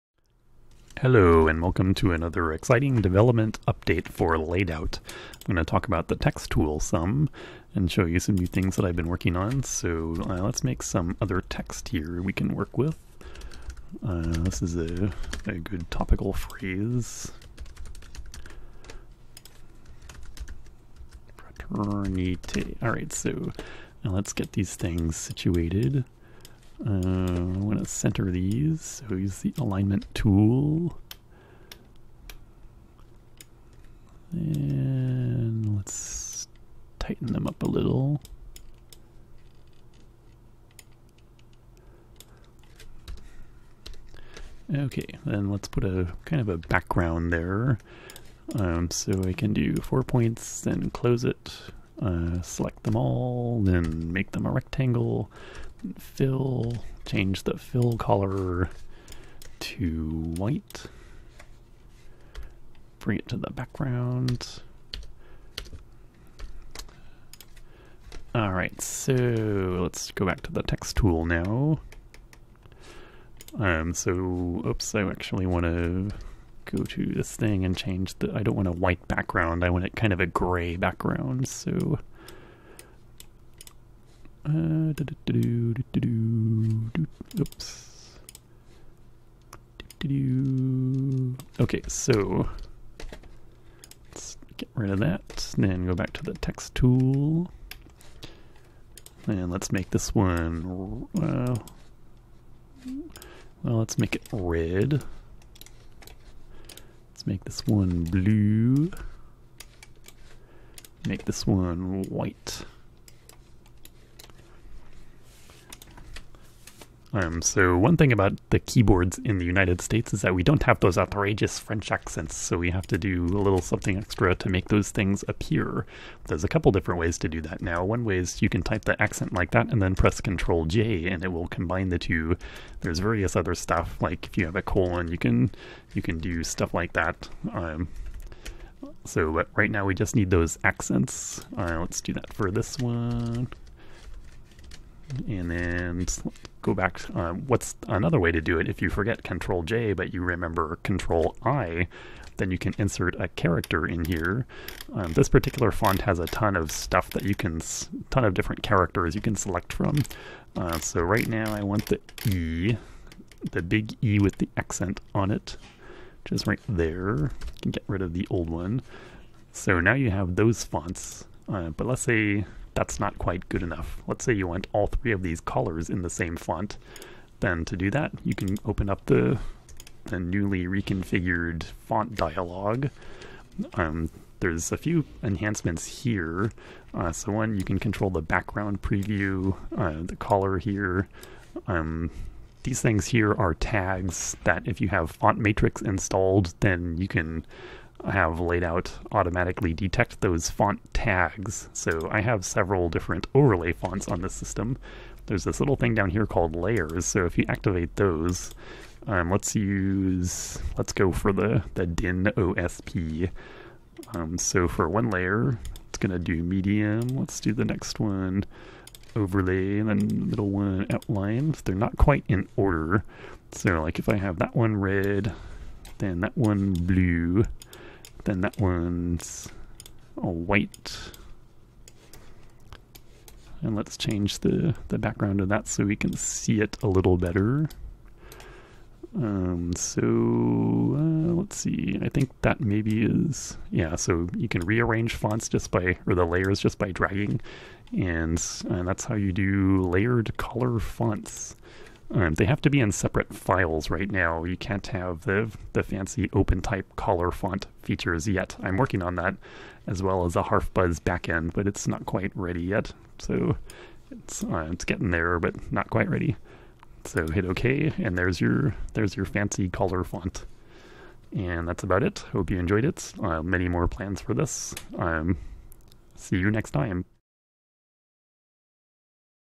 Laidout's Text Tool with layered color fonts
Here's another development update about what's been going on in Laidout coding. You can construct a layered font, where each file is a different colored layer in the final font. It's my hope to someday support opentype based color fonts, but this will have to do for now. Also new is the ability to grab Fontmatrix font tags (if you have Fontmatrix installed), and use them to narrow a search for fonts.

You can learn more about color fonts at these places (for instance), and also download fonts that I used in the video: